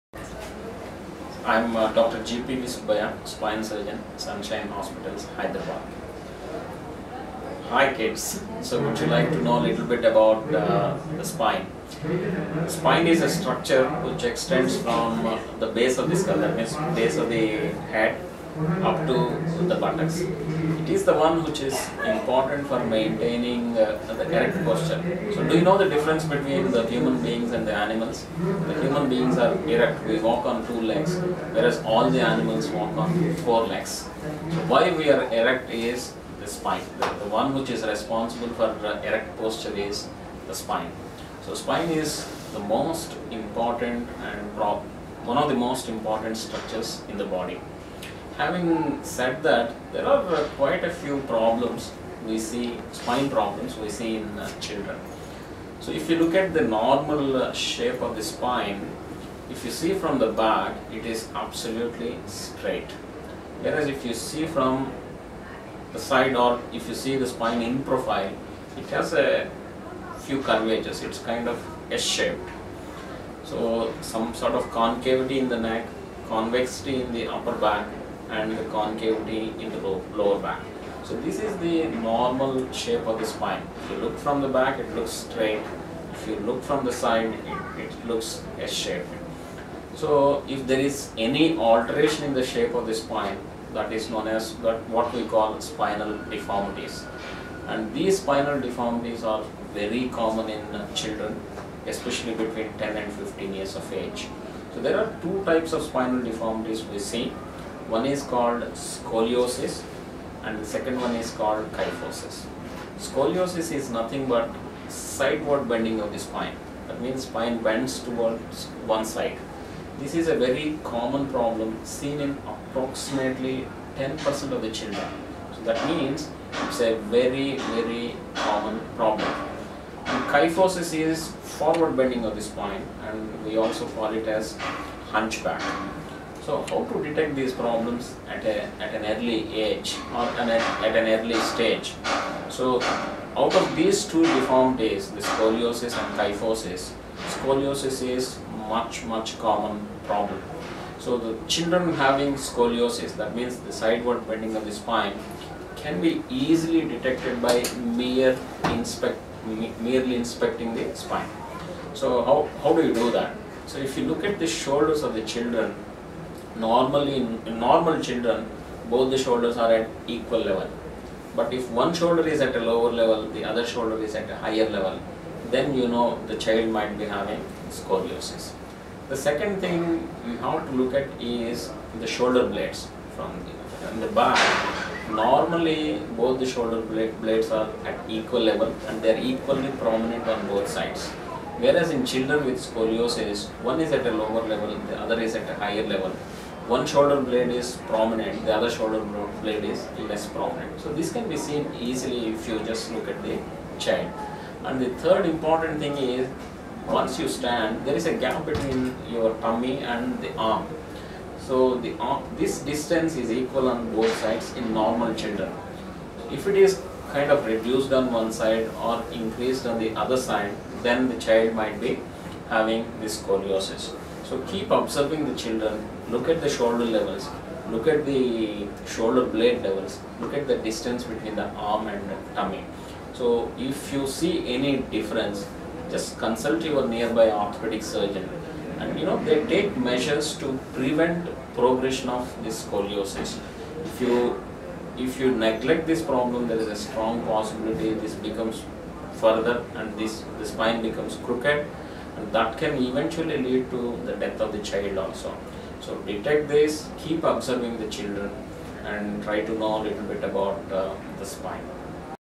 Dr.G.P.V.Subbaiah,spine surgeon on minimally invasive spine surgery
Dr.G.P.V.Subbaiah,spine surgeon,Sunshine hospitals,Secunderabad, discussing the advantages of minimally invasive spine surgery over conventional spine surgery and strategies to prevent spine problems